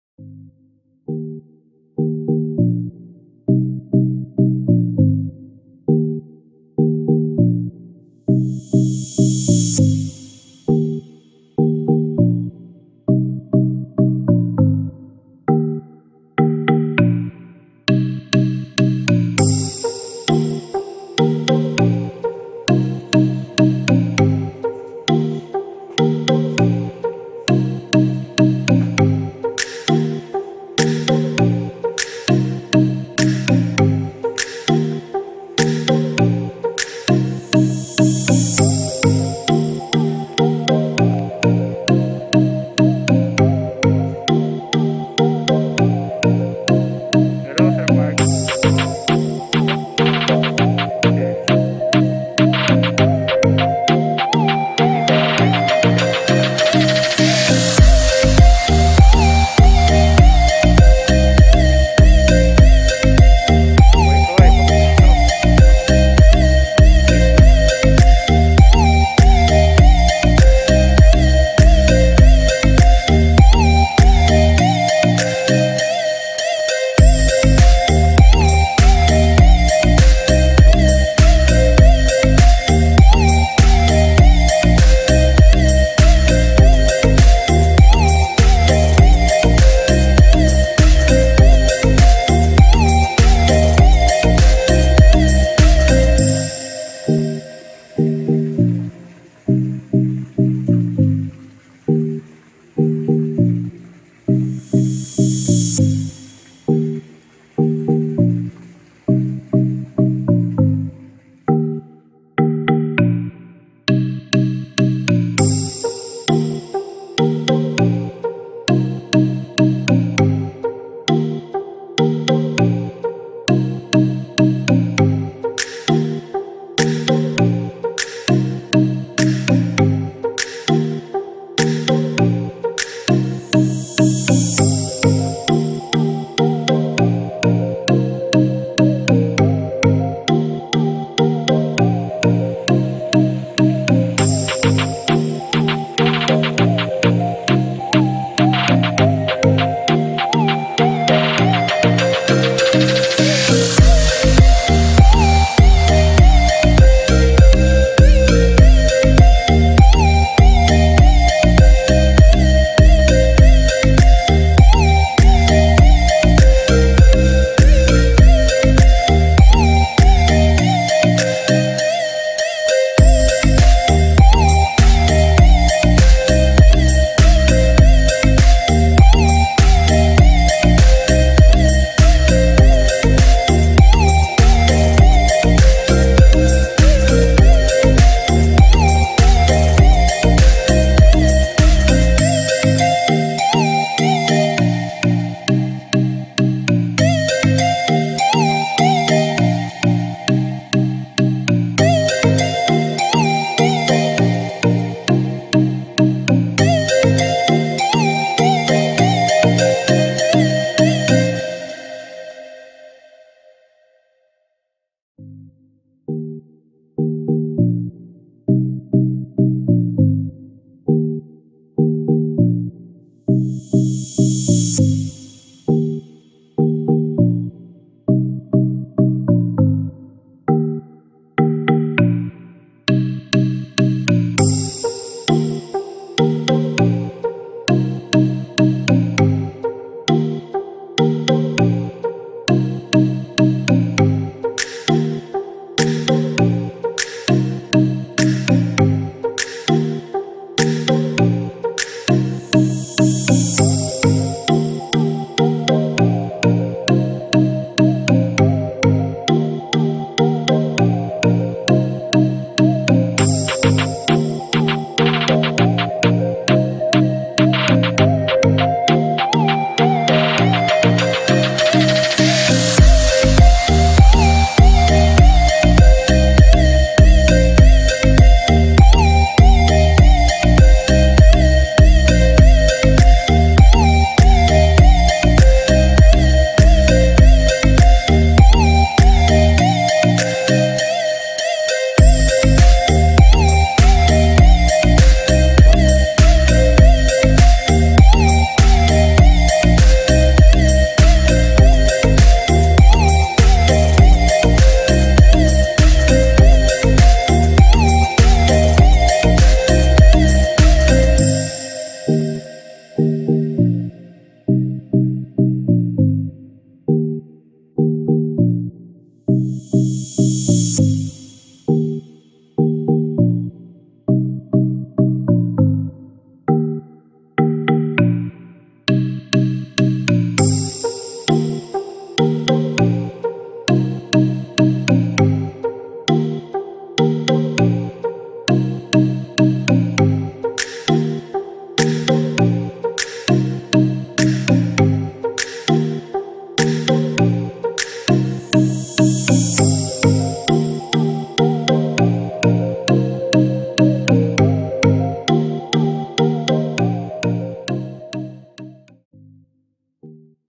Manabat Farm Resort
Manabat Farm Resort  @ Magalang Pampanga Sounds and Lights setup